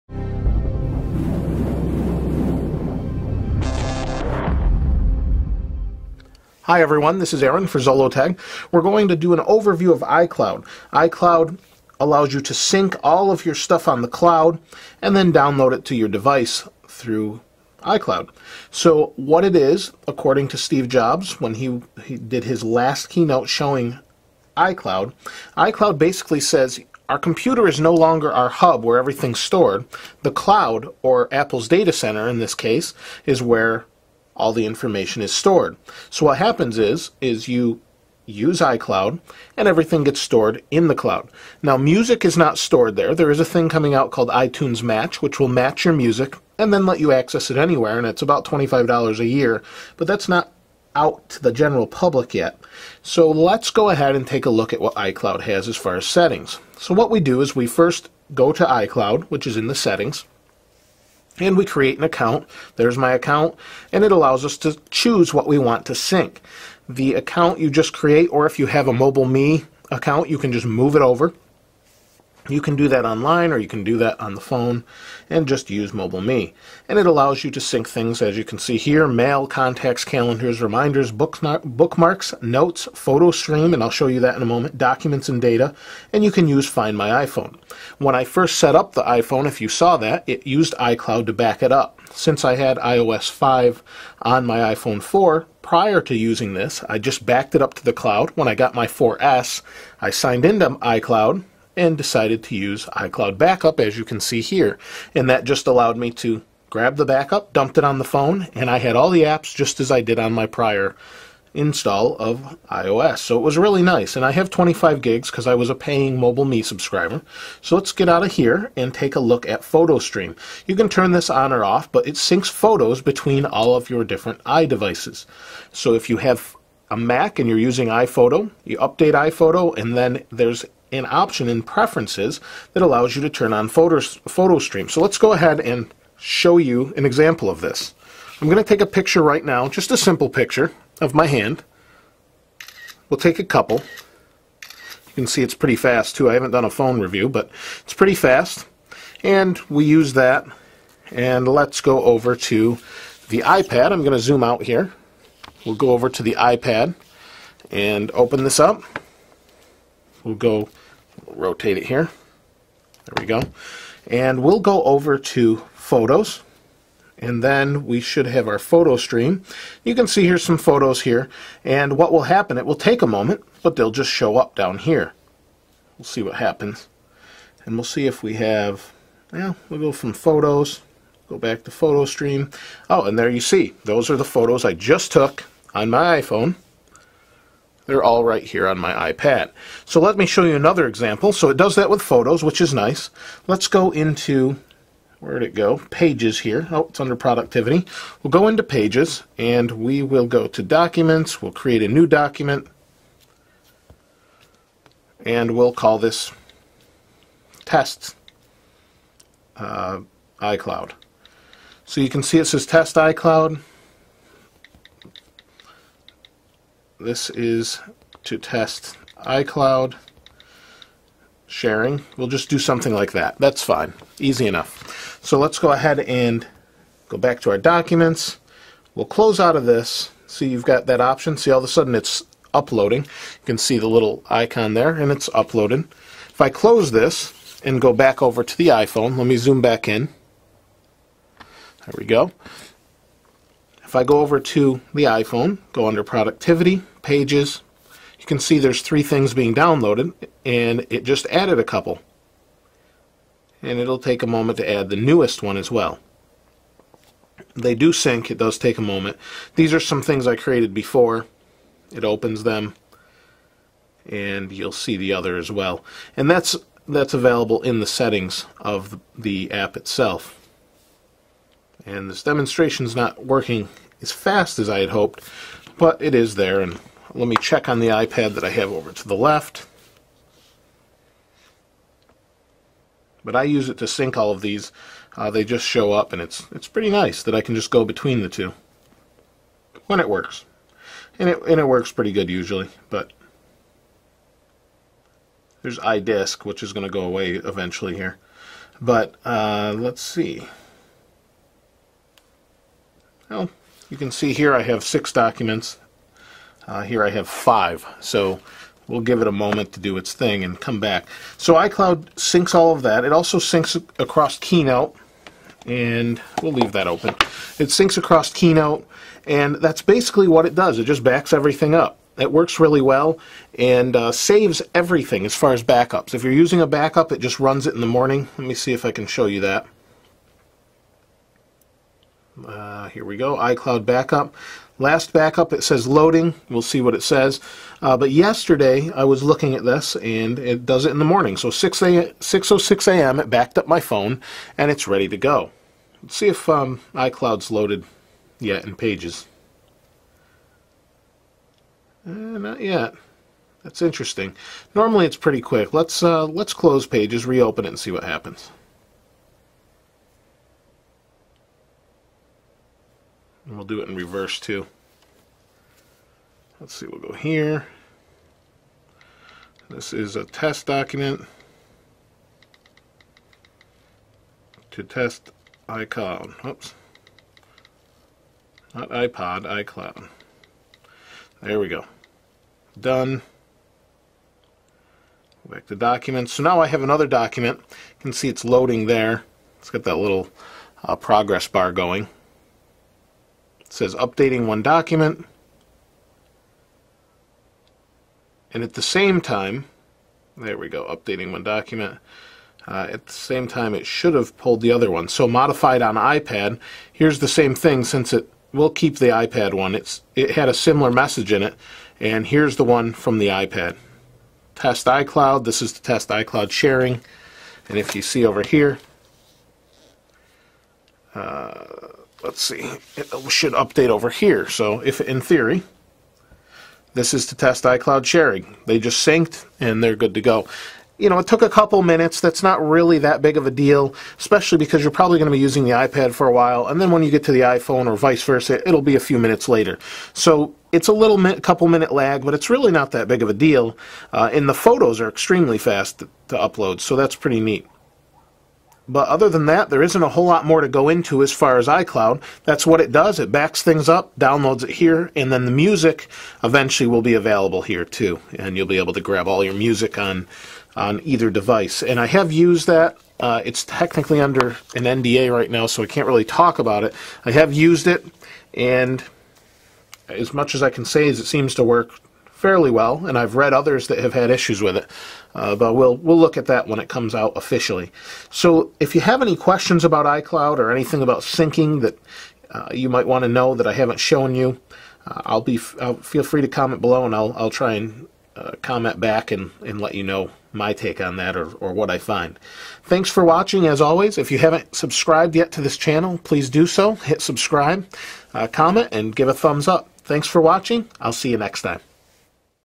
iCloud Overview
I show what iCloud is all about. I do a demo of photo stream and show what it's like to sync a document as well.  Check out the video for all the details and be sure to comment on your experience with iCloud.